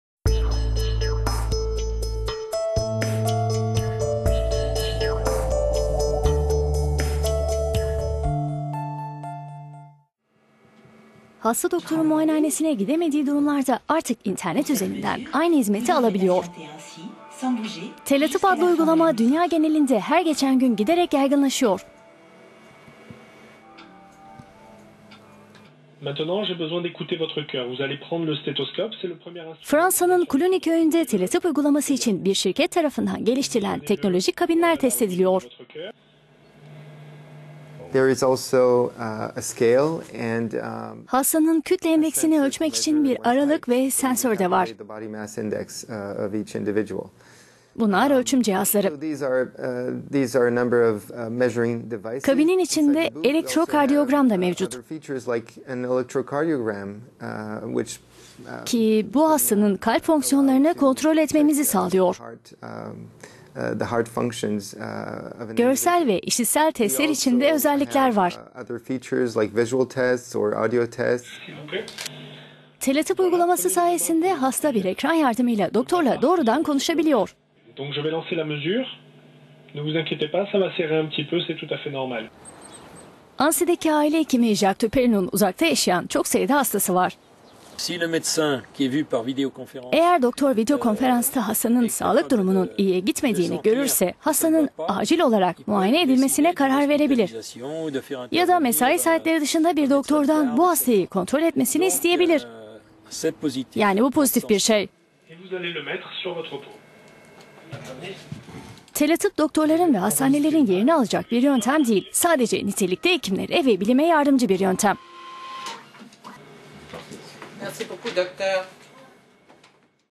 Teletıp uygulamasıyla doktorunuz evinize geliyor - hi-tech
Hasta doktorun muayenehanesine gidemediği durumlarda artık internet üzerinden aynı hizmeti alabiliyor. "Teletıp" adlı uygulama dünya genelinde her geçen gün yaygınlaşıyor.

Fransa'nın Cluny köyünde "teletıp" uygulaması için H4D adlı şirket tarafından geliştirilen teknolojik kabinler test ediliyor: "Hastanın kütle endeksini ölçmek için bir aralık ve sensör de var. Bunlar ölçüm cihazları. Kabinin içinde elektrokardiyogram da mevcut ki bu hastanın kalp fonksiyonlarını kontrol etmemizi sağlıyor. Ayr…